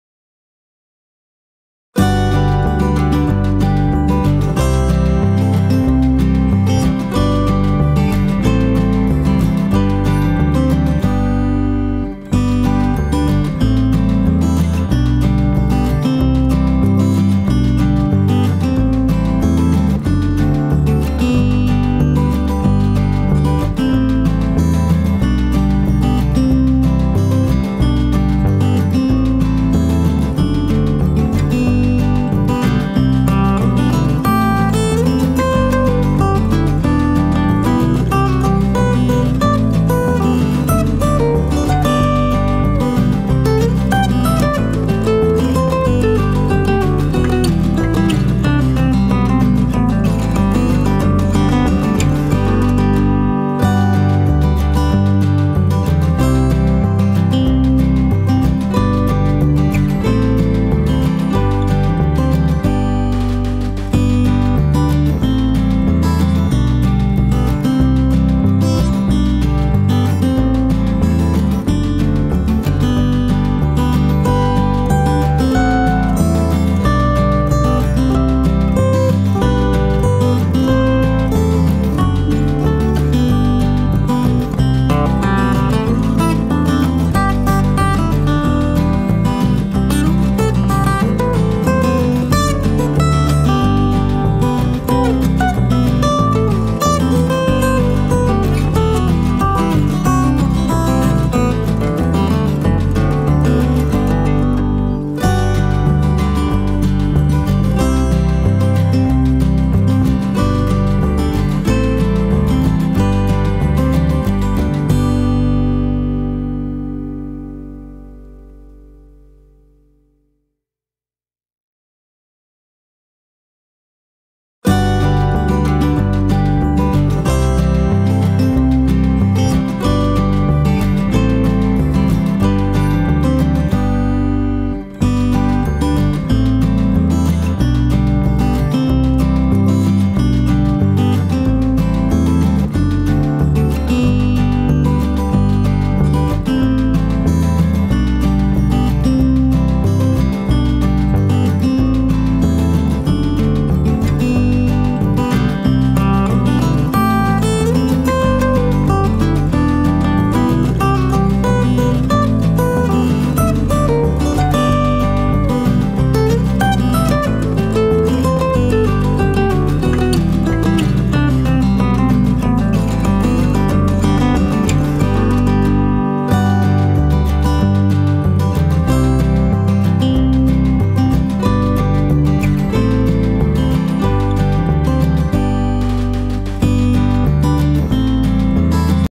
Simple House of Scenery Drawing ❤️ Easy To Draw 🏠 Art Pencil
In this video I will show you simple house of scenery drawing very easily.